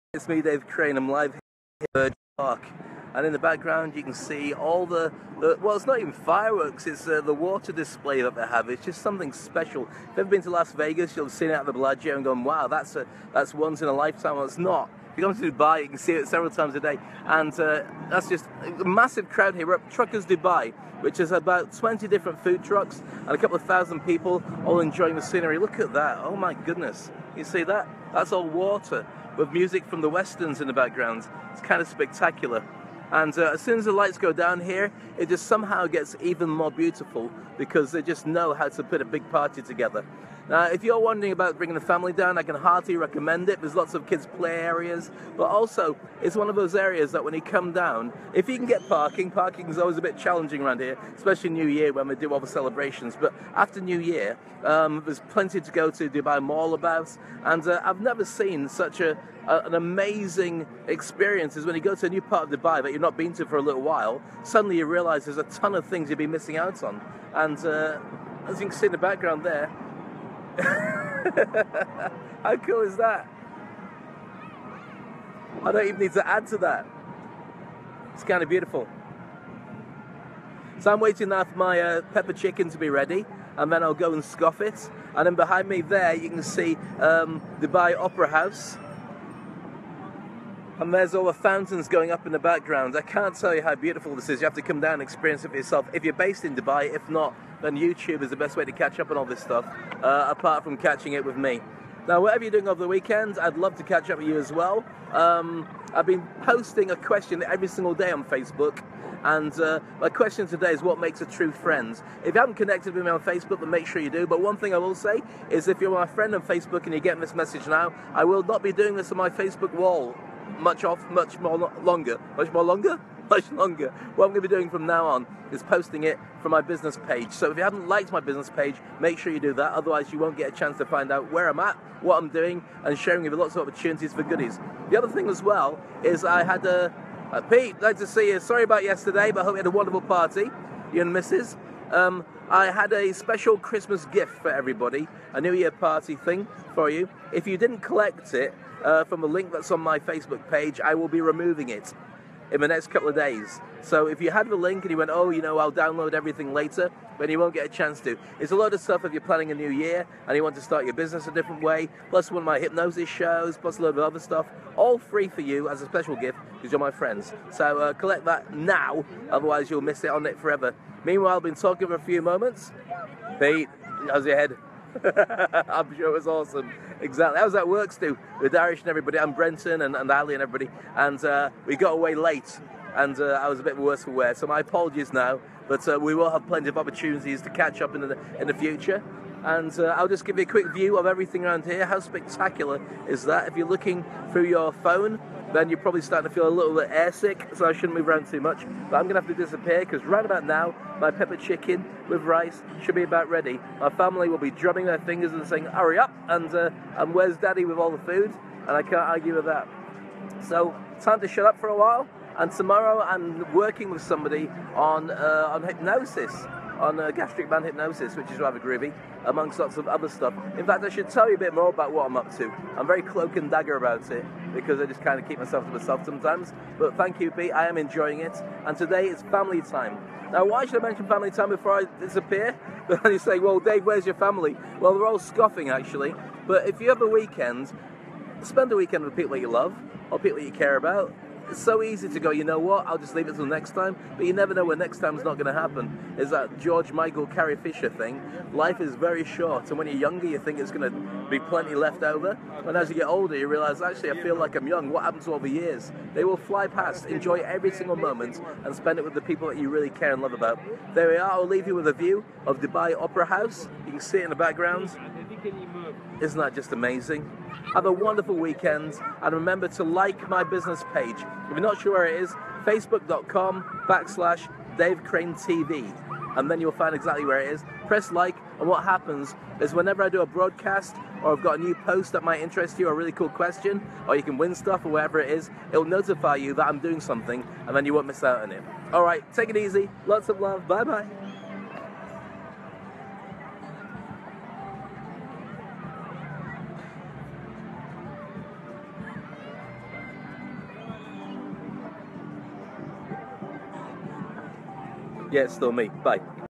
Truckers Dubai The best way to enjoy the weekend
Work-life balance should be at the heart of an entrepreneurs schedule. Truckers Dubai in Burj park is an awesome place to take the family.